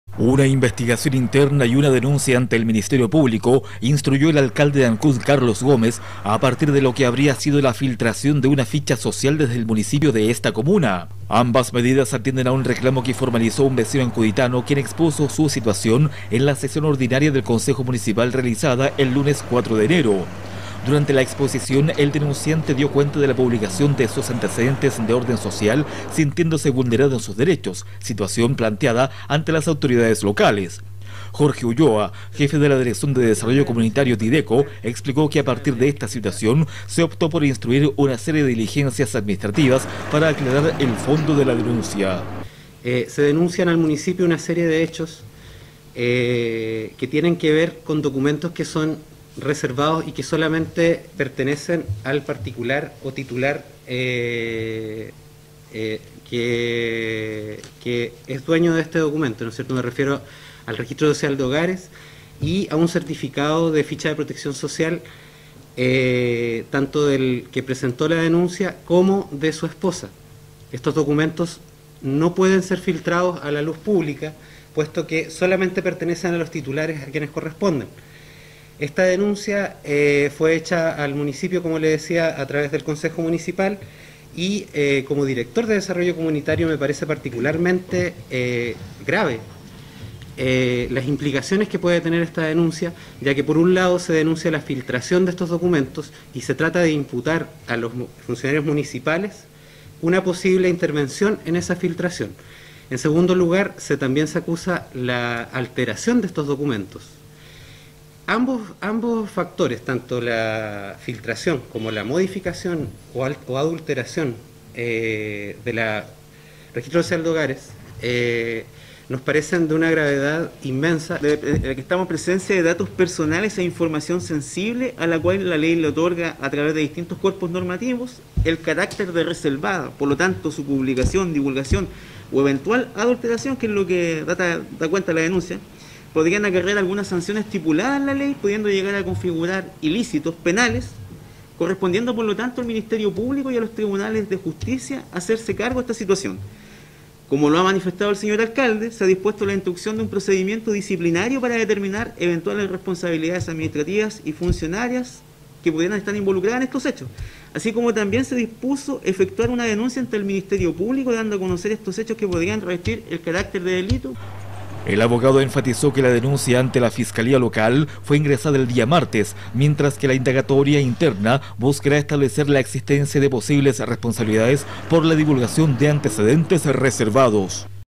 Ancud: abren investigación e ingresan denuncia por filtración de ficha social desde el municipio
Una investigación interna y una denuncia ante el Ministerio Público instruyó el alcalde de Ancud, Carlos Gómez, a partir de lo que habría sido la filtración de una ficha social desde el municipio de esta comuna.
Ambas medidas atienden a un reclamo que formalizó un vecino ancuditano quien expuso su situación en la sesión ordinaria del Concejo Municipal realizada el lunes 4 de enero.
Durante la exposición el denunciante dio cuenta de la publicación de sus antecedentes de orden social sintiéndose vulnerado en sus derechos, materia planteada ante las autoridades locales.
Jorge Ulloa, jefe de la Dirección de Desarrollo Comunitario (Dideco) explicó que a partir de esta situación se optó por instruir una serie de diligencias administrativas para aclarar el fondo de la denuncia.
“Se denunciaron hechos que tienen que ver con documentos reservados y que solamente pertenecen al particular que es dueño de este documento, me refiero al Registro Social de Hogares y a un certificado de ficha de protección social tanto del que presentó la denuncia como del de su esposa, estos documentos no pueden ser filtrados a la luz pública”, aseguró el funcionario municipal.
Ulloa recalcó que la denuncia encierra una serie de consecuencias donde se deja entrever tanto la filtración de documentos privados como así también “se trata de imputar a los funcionarios municipales una posible intervención en esa filtración y en segundo lugar se acusa la alteración de estos documentos, ambos factores, tanto la filtración como adulteración del registro social de hogares nos parecen de una gravedad inmensa”.
Por su parte el asesor jurídico municipal, Oscar Díaz, confirmó el tenor de la denuncia y el emprendimiento de acciones de carácter legal. El jurista mencionó que “como lo ha manifestado el señor alcalde, se ha dispuesto de un procedimiento disciplinario para determinar eventuales responsabilidades administrativas y funcionarias que pudieran estar involucradas en estos hechos y también se dispuso efectuar una denuncia en el Ministerio Público dando a conocer estos hechos que podrían revestir el carácter de delito”.
El abogado enfatizó que la denuncia ante la Fiscalía Local fue ingresada el día martes mientras que la indagatoria interna buscará establecer la existencia de posibles responsabilidades por la divulgación de antecedentes reservados.